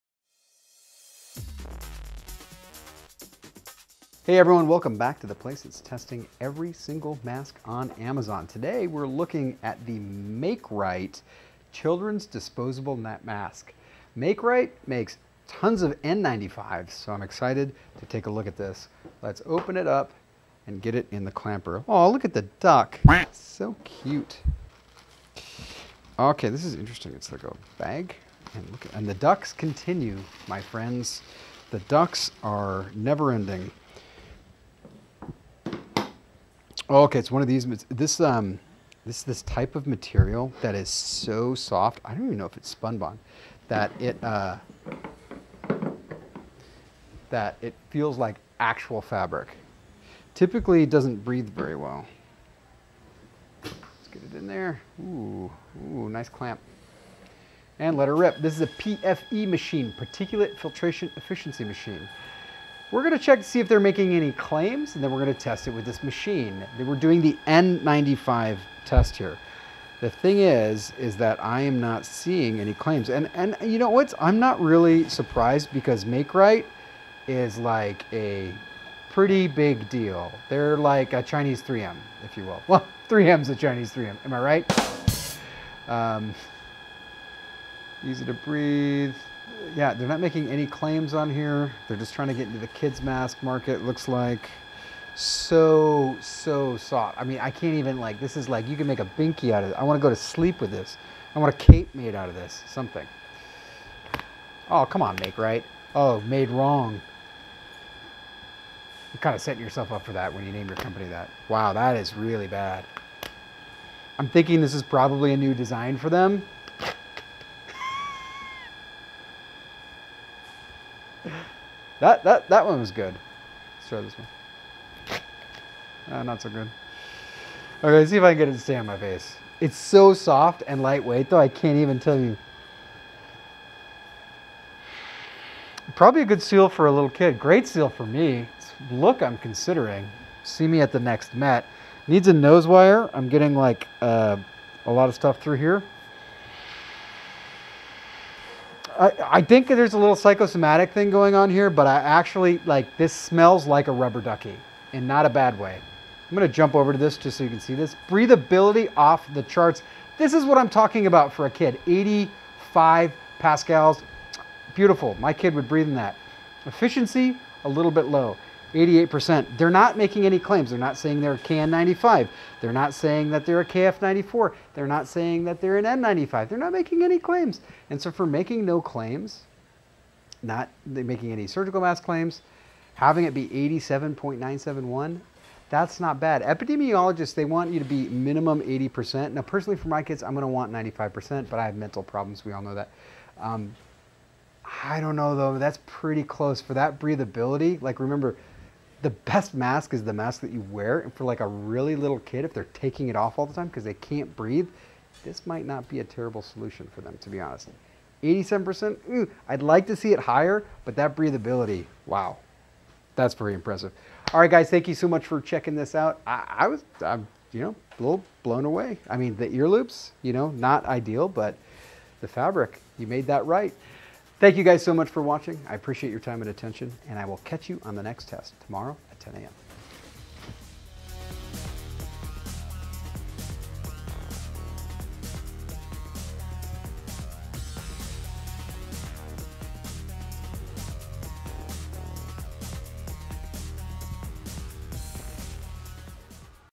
The ducks are never-ending - Makrite Children's Disposable Face Mask 880S Review
Makrite Children's Disposable Face Mask 880S manufactured in China by Dongguan Sure On Plastic and Fiber Products

Lloyd Armbrust
13501 Immanuel Rd